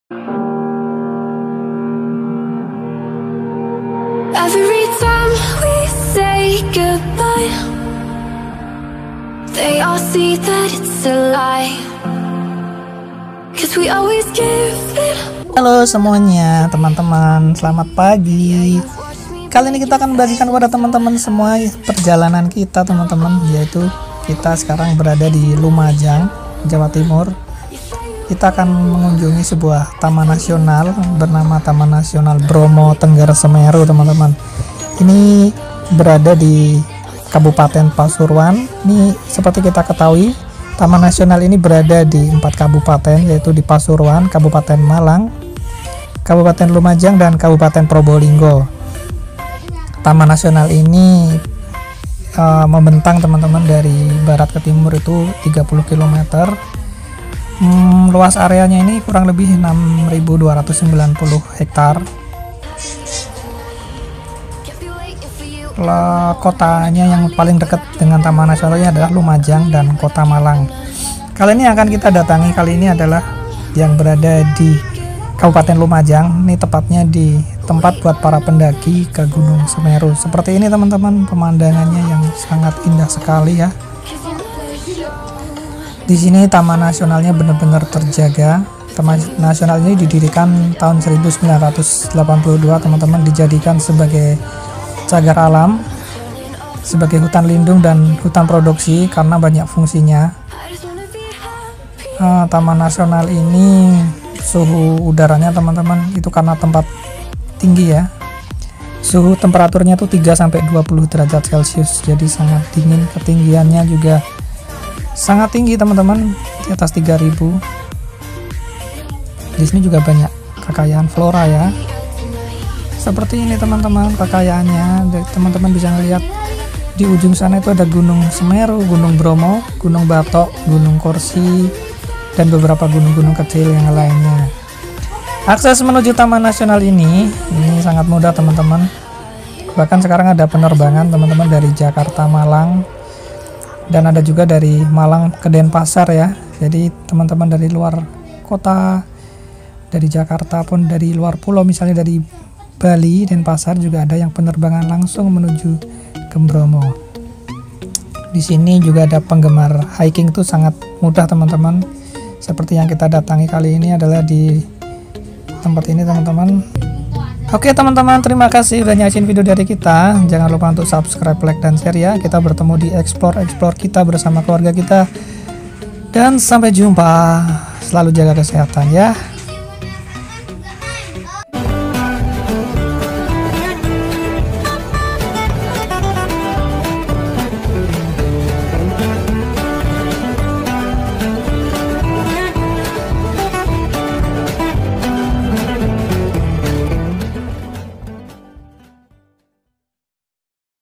WOW!! Indahnya Gunung Bromo di Taman Nasional Bromo Tengger Semeru - Kabupaten Lumajang Jawa Timur
Kali ini kita akan explore betapa indahnya Gunung Bromo dari taman nasional Tengger Semeru di Lumajang Jawa Timur. Pada Hari ini Minggu, 14 Februari 2021.